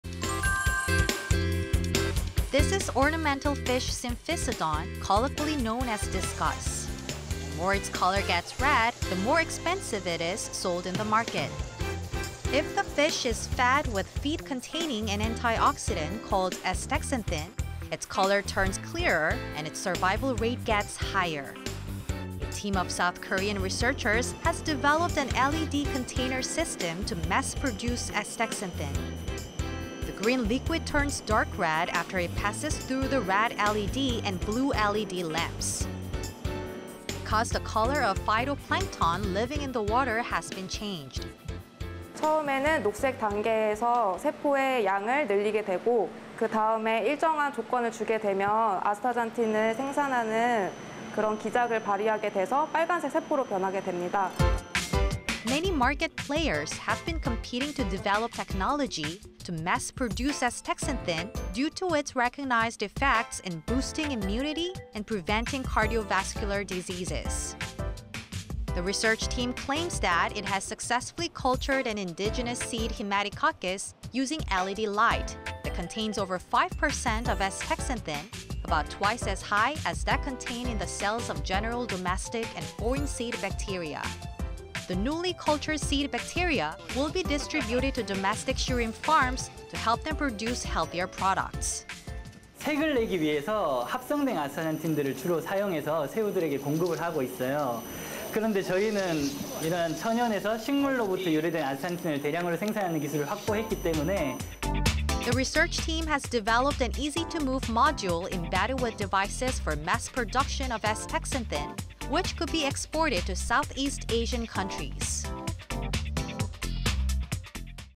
[K-SCIENCE] Mass Production of Astaxanthin / YTN KOREAN
This is ornamental fish Symphysodon, colloquially known as discus. The more its color gets red, the more expensive it is sold in the market.

If the fish is fed with feed containing an antioxidant called astaxanthin, its color turns clearer and its survival rate gets higher.

A team of South Korean researchers has developed an "LED container system" to mass produce astaxanthin.

The green liquid turns dark red after it passes through the red LED and blue LED lamps.

Because the color of phytoplankton living in the water has been changed.

"At the initial stage, the liquid looks green increasing the number of cells. But it turns red under specific circumstances as it acquires mechanism to produce astaxanthin."

Many market players have been competing to develop technology to mass produce astaxanthin due to its recognized effects in boosting immunity and preventing cardiovascular diseases.

The research team claims that it has successfully cultured an indigenous seed haematococcus using LED light, that contains over five percent of astaxanthin, about twice as high as that contained in the cells of general domestic and foreign seed bacteria.

The newly cultured seed bacteria will be distributed to domestic shrimp farms to help them produce healthier products.

[Kim Dae-Geun, professor, Chonbuk Nat'l University]
"We are supplying newly synthesized astaxanthin to the shrimp farms as we have developed technology to mass produce astaxanthin originating from plants in nature."

The research team has developed an easy-to-move module embedded with devices for mass production of astaxanthin, which could be exported to Southeast Asian countries.